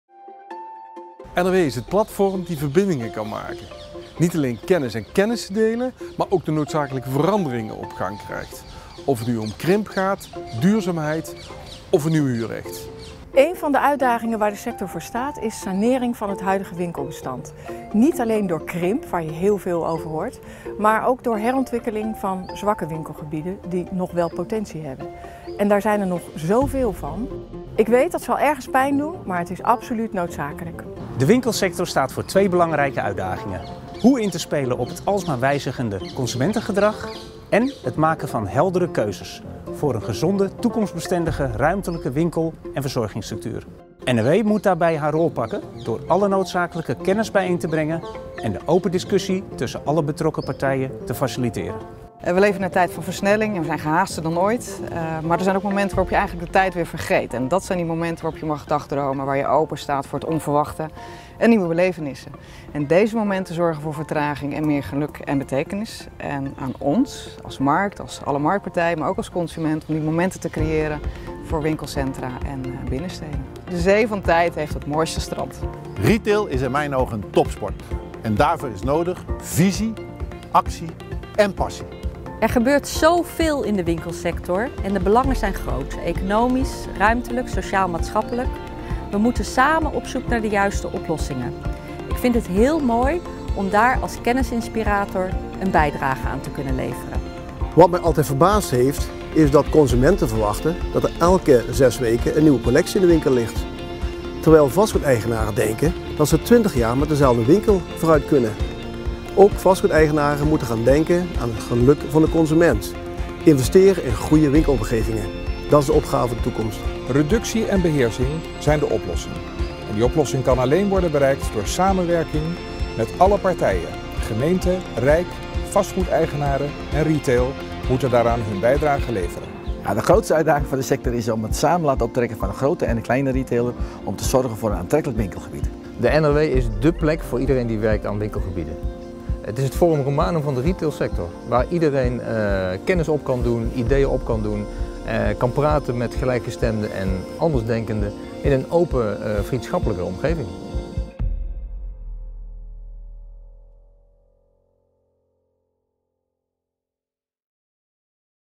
NRW The Movie 2017
Wat zijn de belangrijkste vraagstukken binnen de winkelsector? Hoe kan NRW een bijdrage leveren bij het zoeken naar de juiste oplossingen?